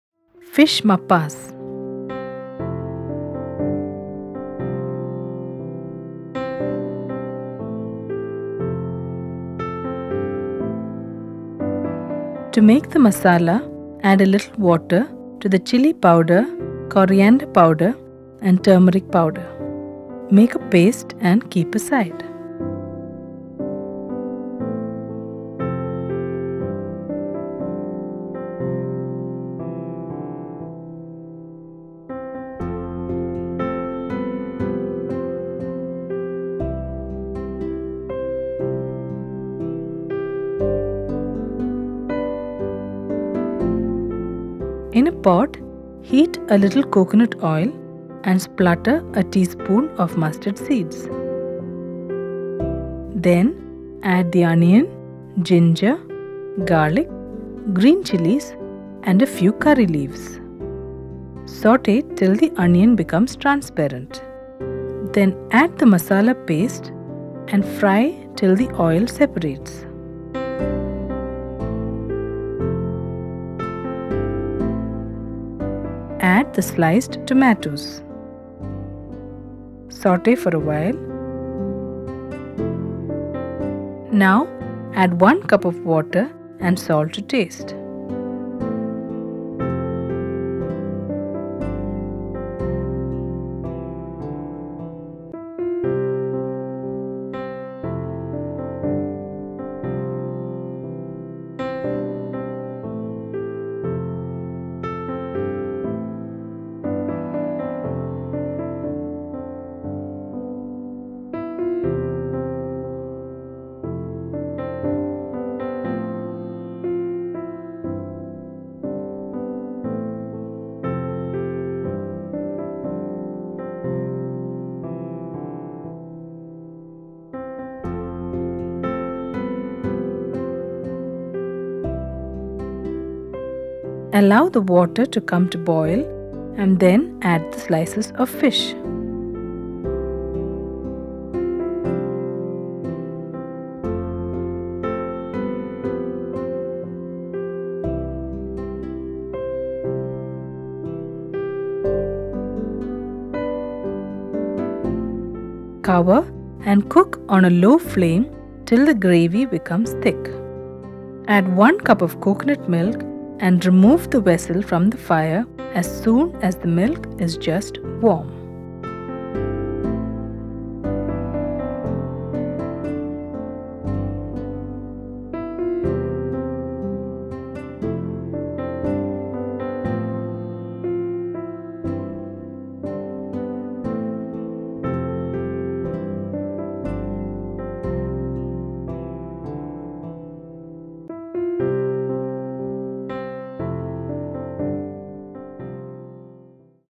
Fish Mappas | Fish Cooked in Coconut Milk | Kerala Non-Vegetarian Dish | Kerala Culinary Tourism
Fish Mappas Kerala Cuisine Recipe